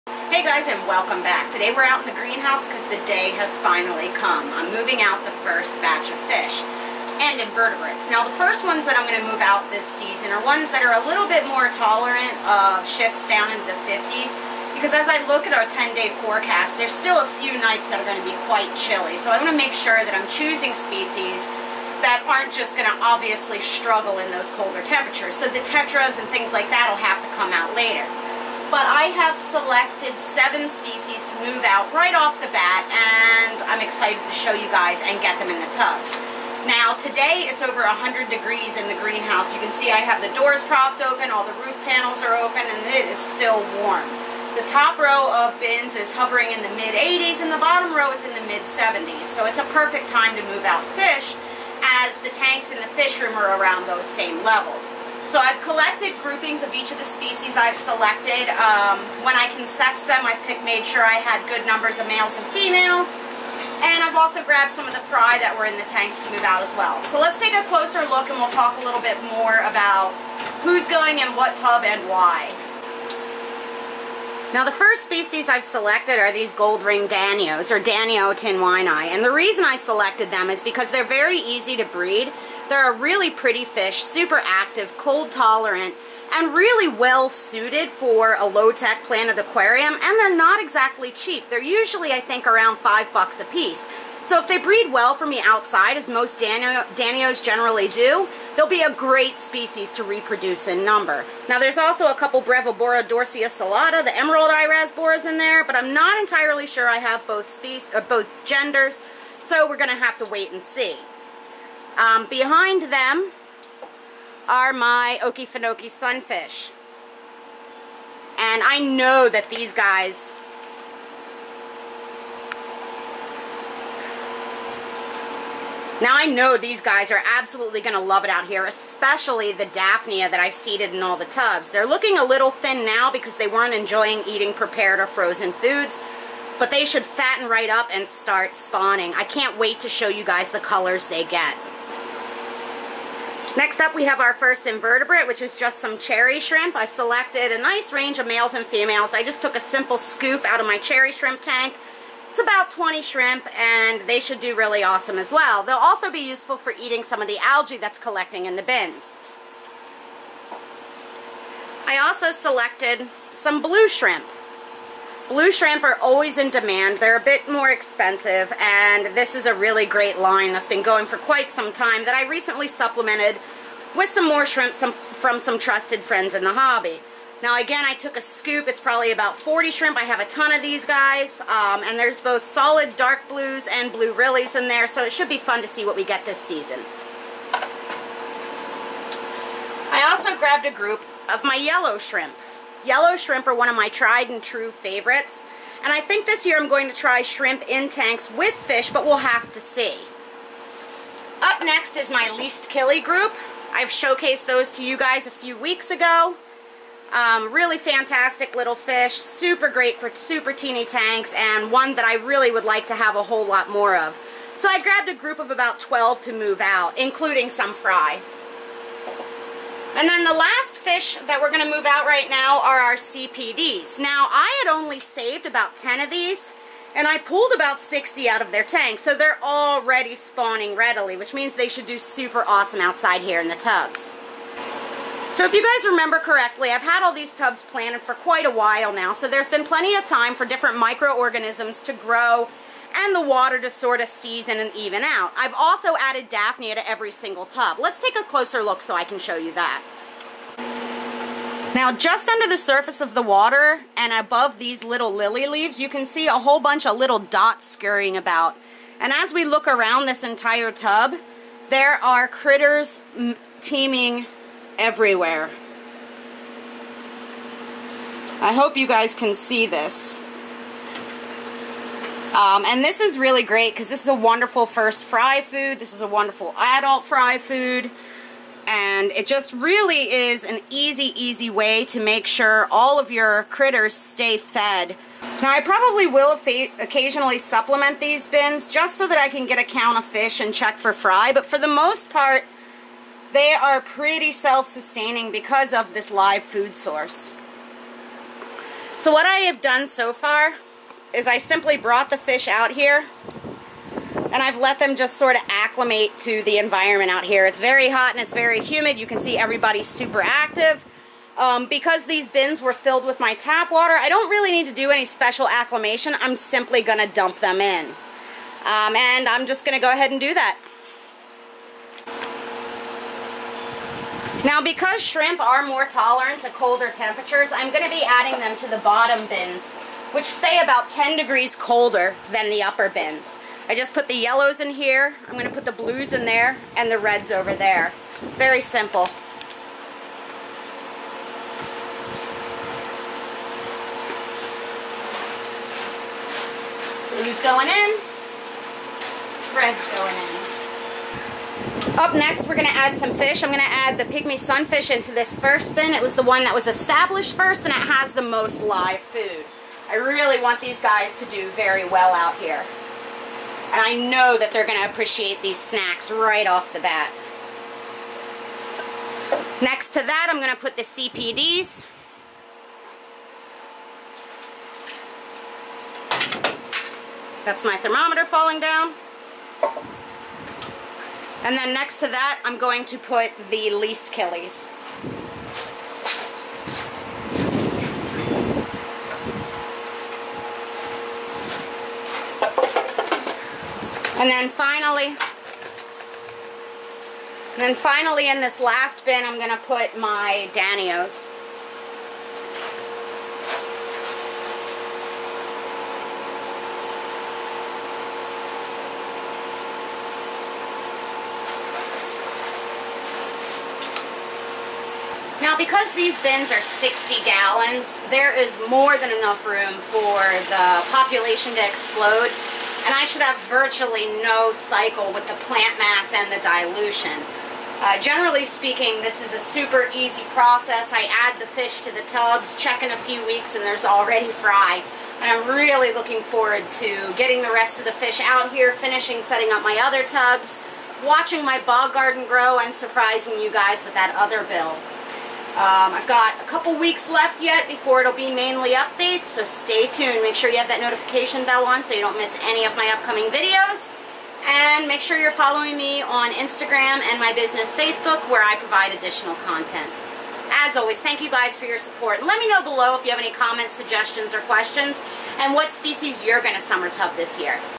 YES! The day has ARRIVED! First fish are IN!
The day has FINALLY arrived! The first batch of fish are in the tubs!